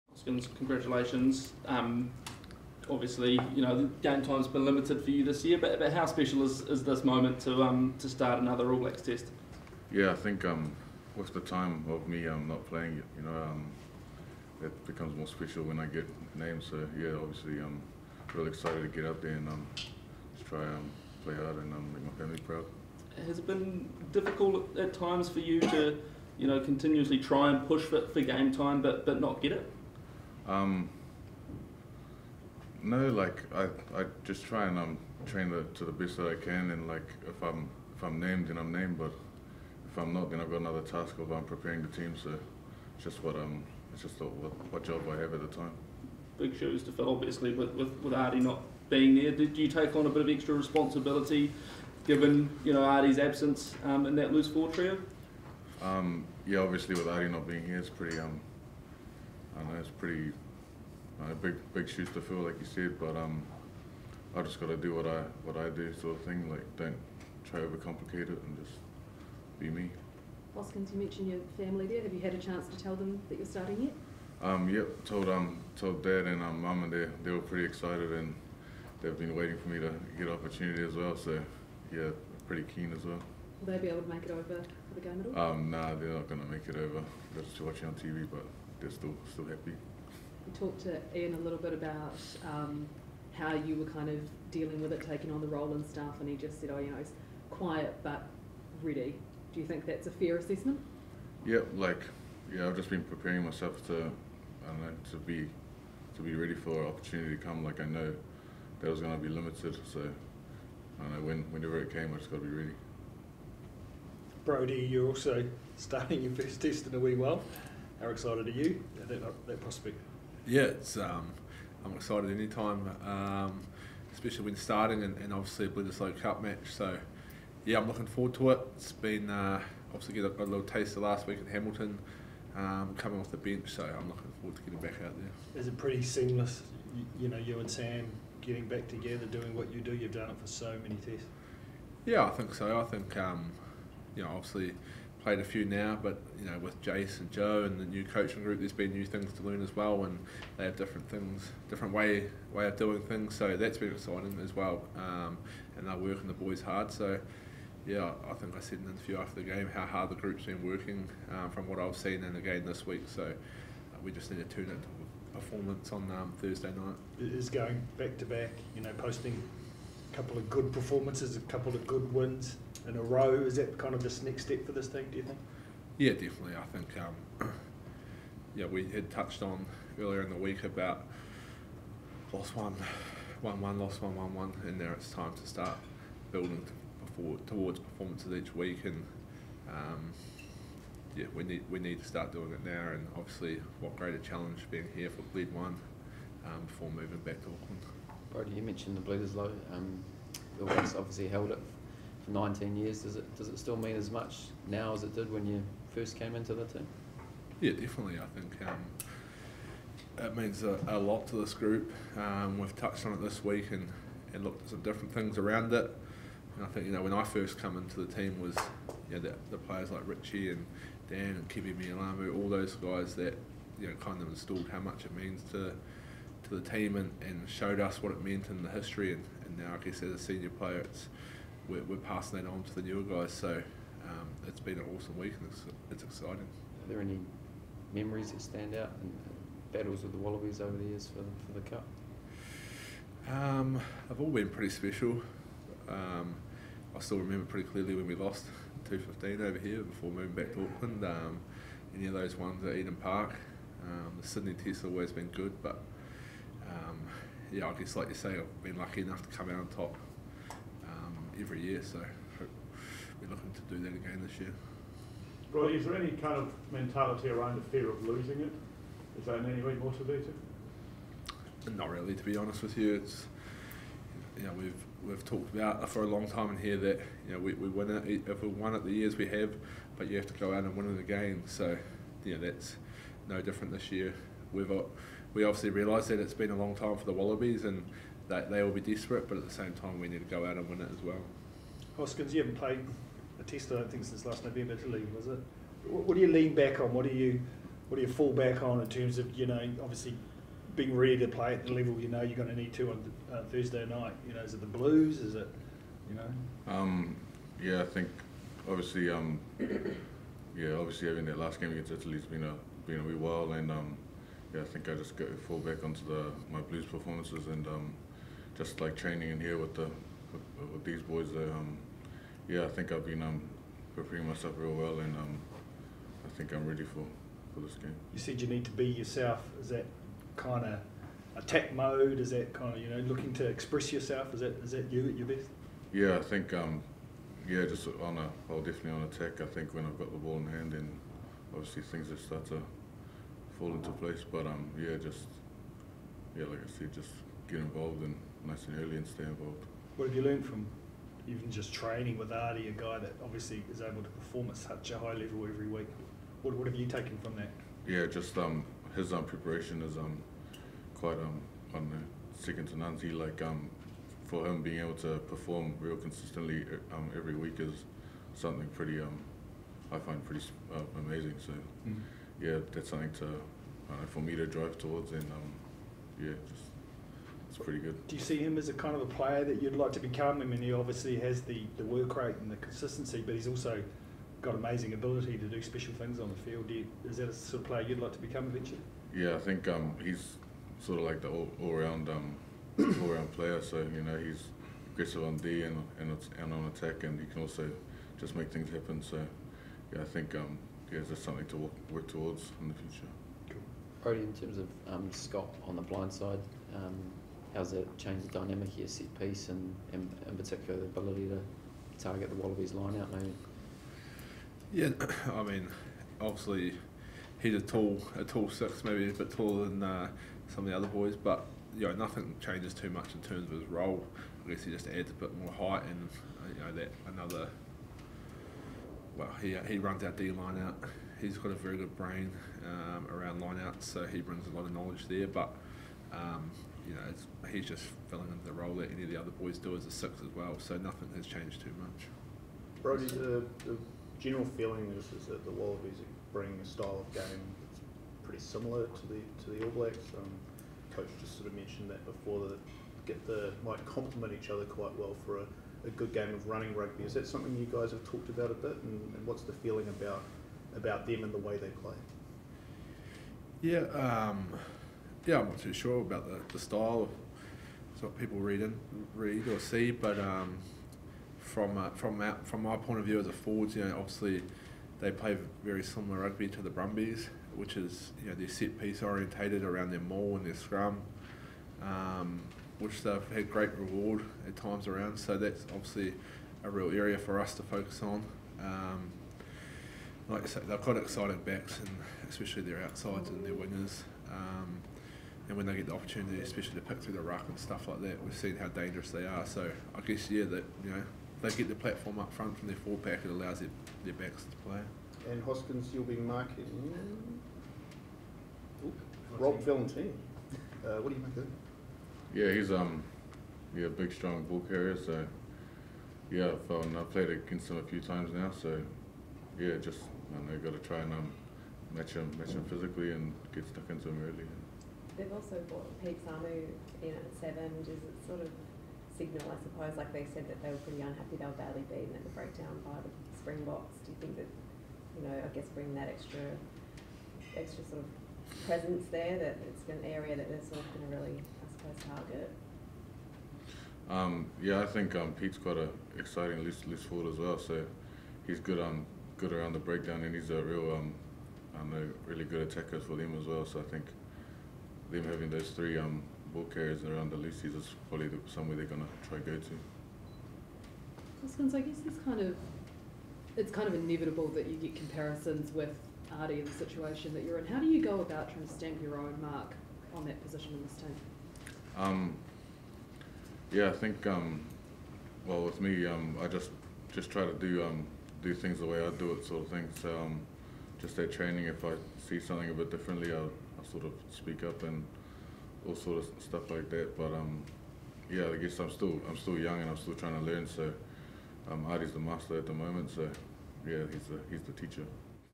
Hoskins Sotutu & Brodie Retallick Press Conference (Melbourne)
Hoskins Sotutu and Brodie Retallick chat to the media ahead of Thursday's Test against Australia at Marvel Stadium.